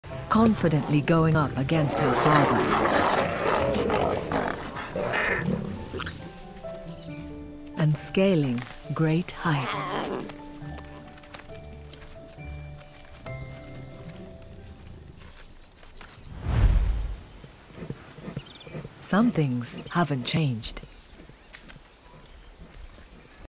Confident Lioness “Maya” Challenges Father
Lioness "Maya" has kept her adventerous cub-like spirit alive!

👉 Watch the full clip on our channel!